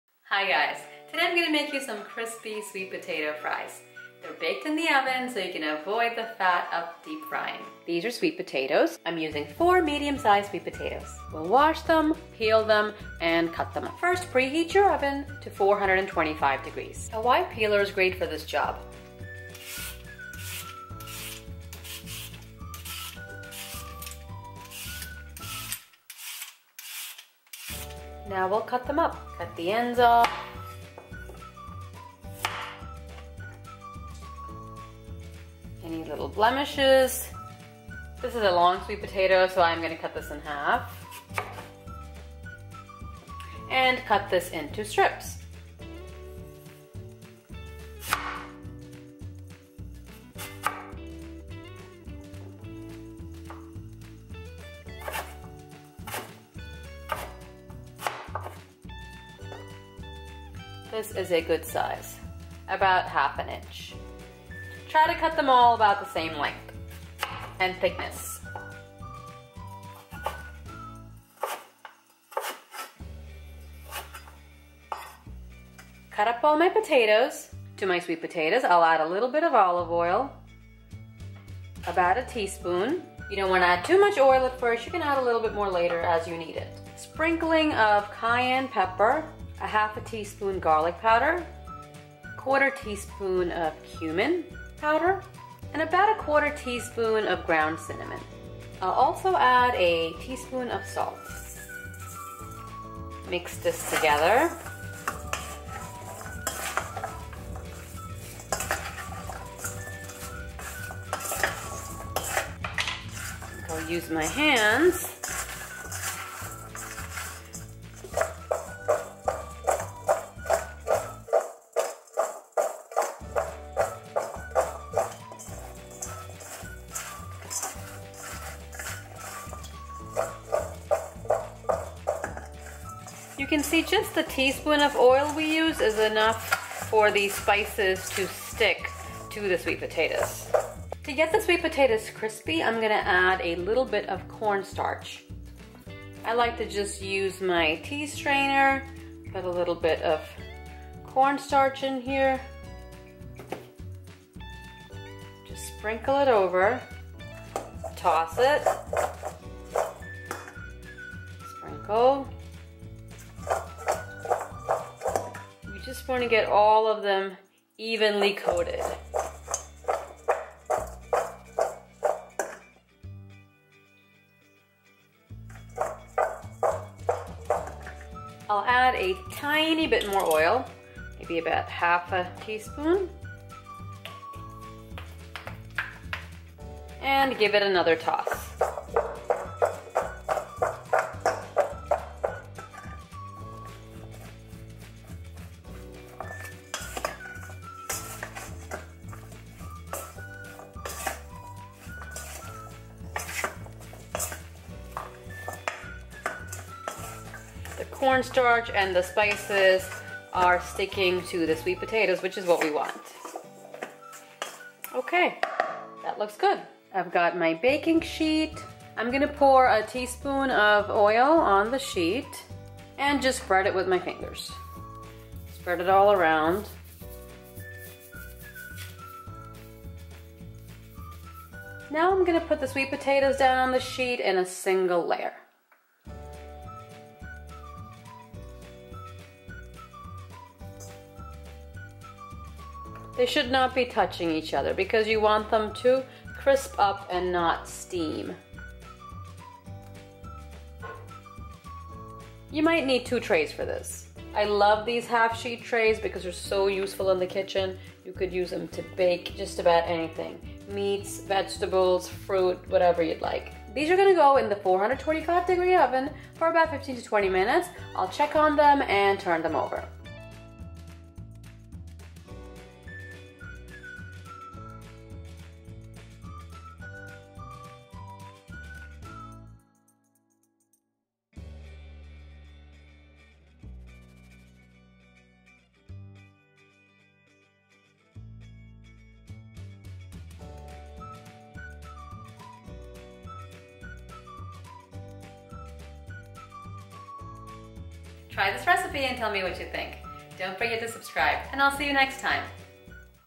Crispy Baked Sweet Potato Fries Recipe
Hi Guys, today I'll show you how to make crispy baked sweet potato fries. These are delicious, healthy and easy to make. I'm using 4 medium sweet potatoes. Peel them and trim the ends. Slice them into even pieces.
Tools used in this recipe:
Baking Sheet
Y Peeler
Toss them with 1 tsp olive oil, pinch of cayenne pepper, 1/2 tsp garlic powder, 1/2 tsp ground cumin, 1/4 tsp ground cinnamon and 1 tsp kosher salt. Mix well. Sift corn starch over sweet potatoes evenly and toss. I used 1 to 2 tbsps. Add 1/2 tsp oil and toss again. Pour 1 tsp olive oil on a baking sheet and spread evenly. Arrange sweet potatoes in a single layer on baking sheet. You may need to use 2 sheets. Bake at 425 degrees for 15 minutes. Remove from oven and turn the potatoes over. Bake for another 10-15 mins until crispy. Enjoy plain or with ketchup. These are a delicious and healthy way to eat sweet potatoes.